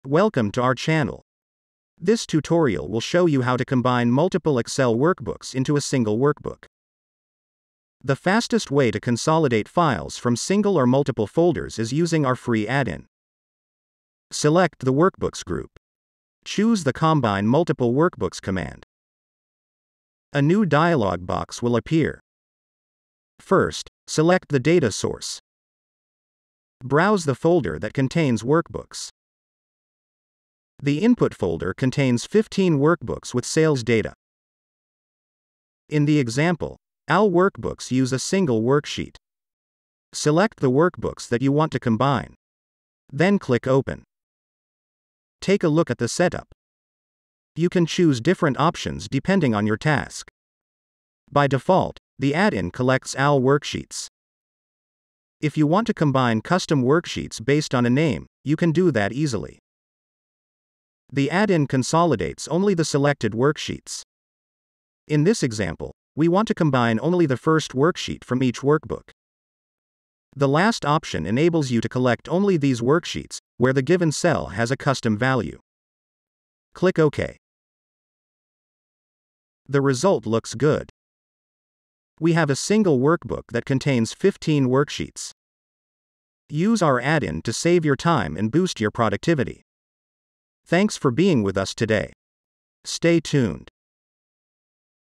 Combine Multiple Excel Files into ONE - (Merge data from Folder)
How to Combine Multiple Excel Files into ONE (Merge data from Folder)

Learn how to combine multiple Excel files into a single workbook in just a few clicks! This tutorial will show you the fastest way to consolidate data from multiple Excel workbooks, whether stored in one or multiple folders.

Chapters:

0:00 Welcome to our Channel
0:03 How to combine multiple Excel Workbooks into one
0:12 Fastest way to consolidate Excel files
0:23 Select the data source (browse for workbooks)
0:45 Setup your preferences
1:16 Combine workbooks into one file
1:26 Final result – all data merged
1:30 Save time & boost productivity

✅ What You'll Learn in This Video:
🔹 How to combine multiple Excel workbooks into one workbook
🔹 The fastest way to merge Excel files from a folder
🔹 How to select specific worksheets to consolidate
🔹 Advanced options for filtering and customizing imported data
🔹 Save time and boost your productivity with our free Excel add-in!